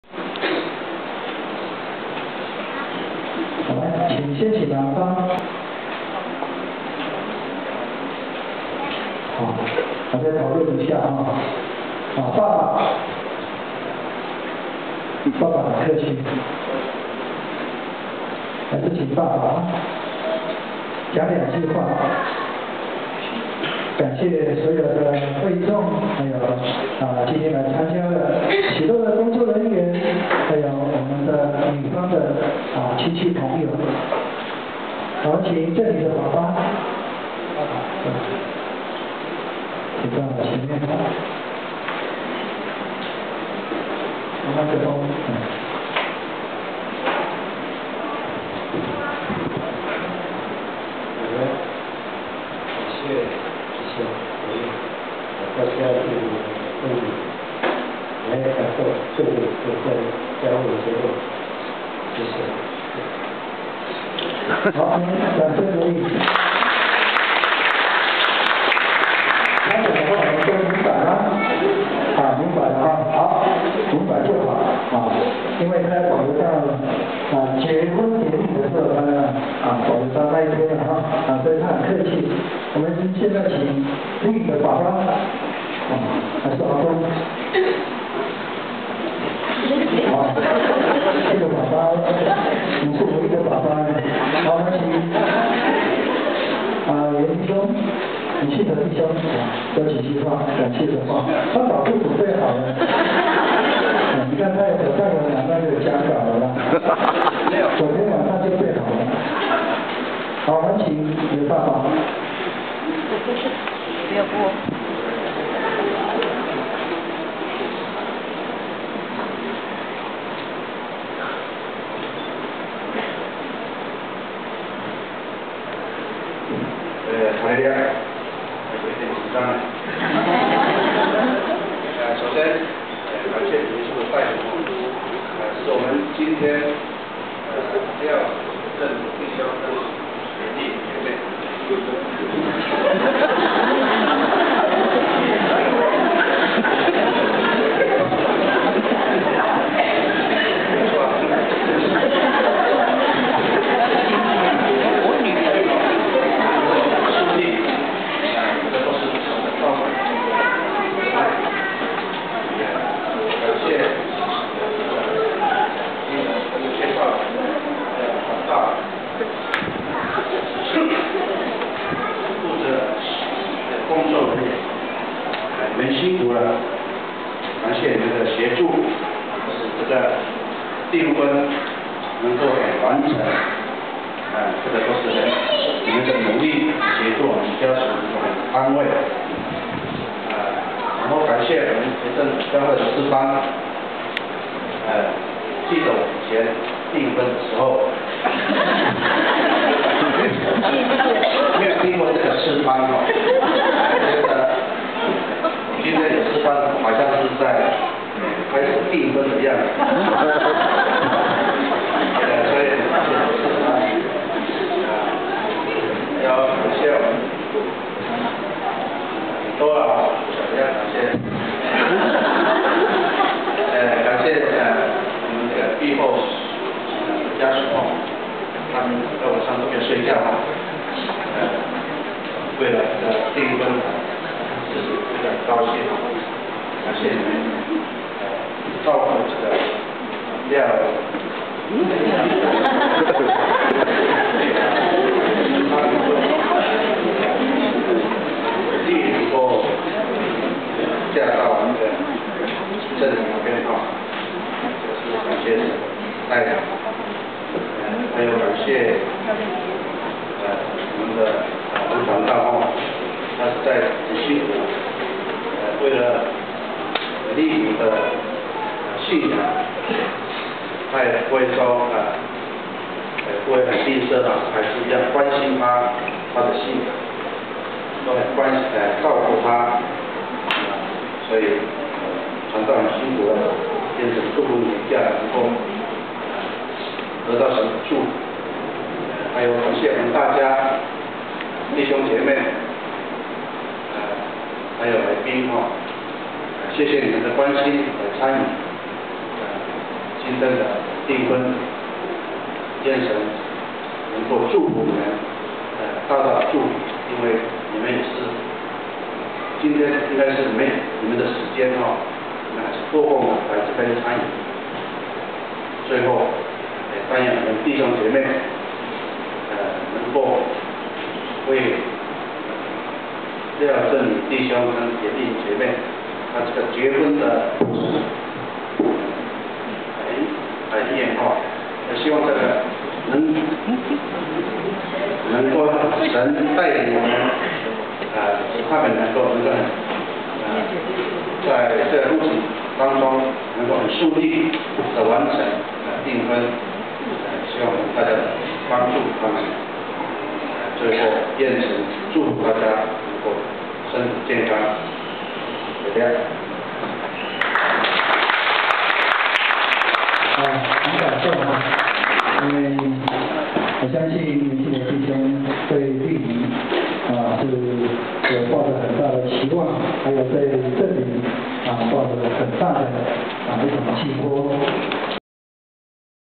泰雅族人與阿美族人訂婚禮~廖政麟與嚴麗潁訂婚禮~廖正發/嚴信德致謝詞
泰雅族人與阿美族人訂婚禮~真耶穌教會碧候教會廖政麟弟兄與前鎮教會嚴麗潁姊妹訂婚禮~雙方主婚人廖正發/嚴信德致謝詞於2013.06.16在前鎮教會二樓大會堂.